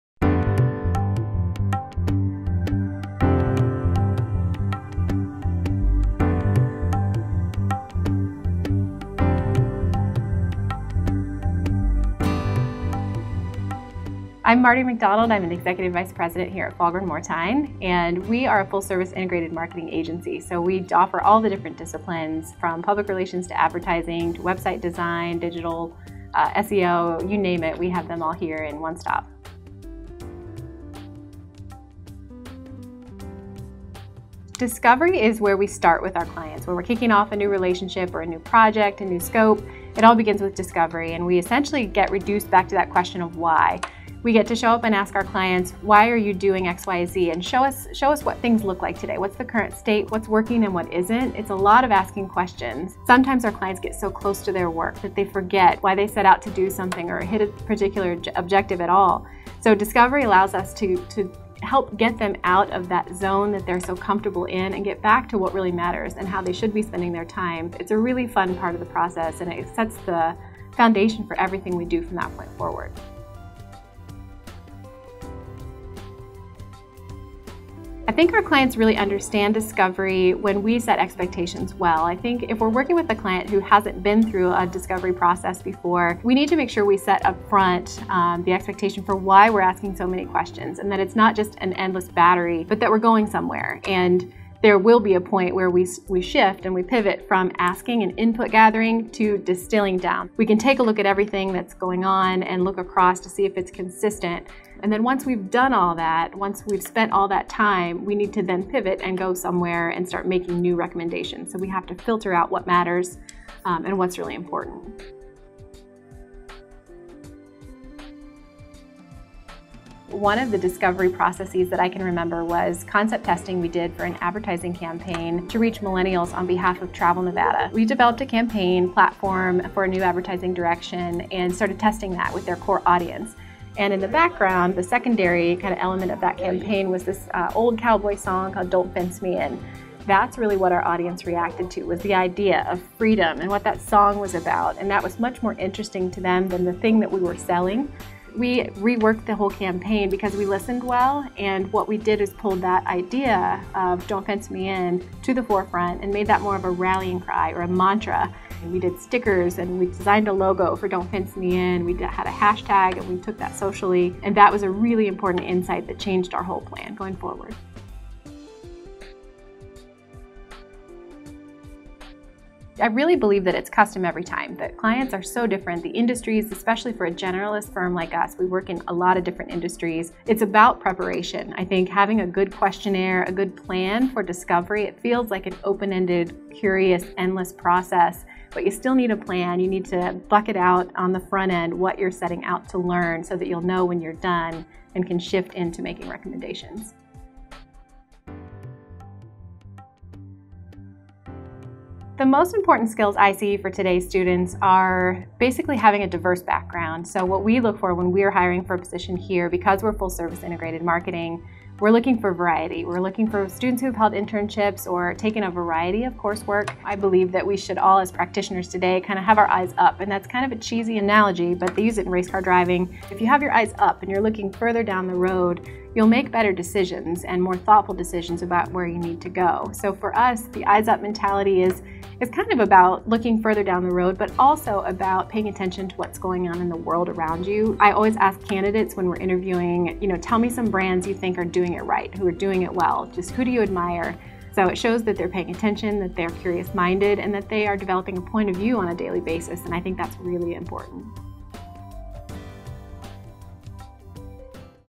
Marty McDonald, Falhgren Mortine: Discovery Process
Marty McDonald, executive vice president at Fahlgren Mortine, explores what the discovery process looks like as part of strategic public relations planning.